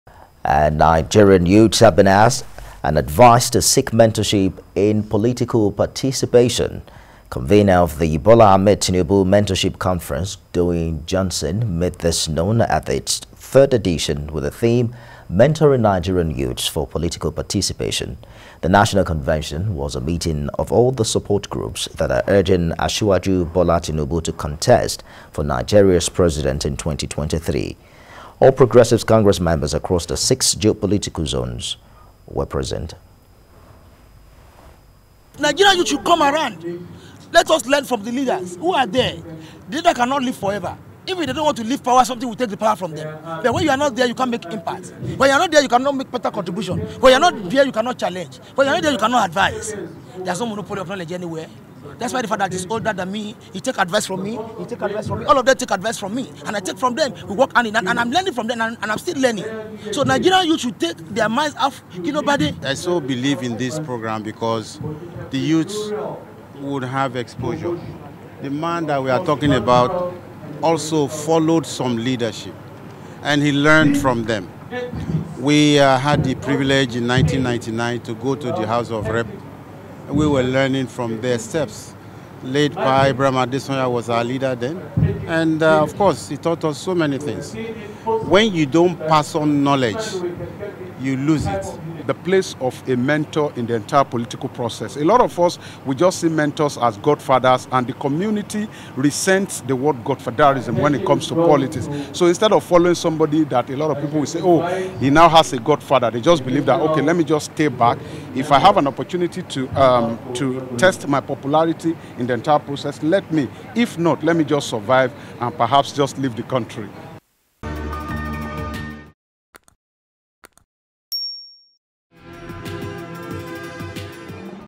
BAT Mentorship Conference Holds 3rd Edition On Mentoring Nigerian Youths For Political Participation
Nigerian youths have been advised to seek mentorship in political participation.

Convener of the Bola Ahmed Tinubu Mentorship Conference, Doyin Johnson made this known at its 3rd edition with the theme, Mentoring Nigerian Youths for Political Participation.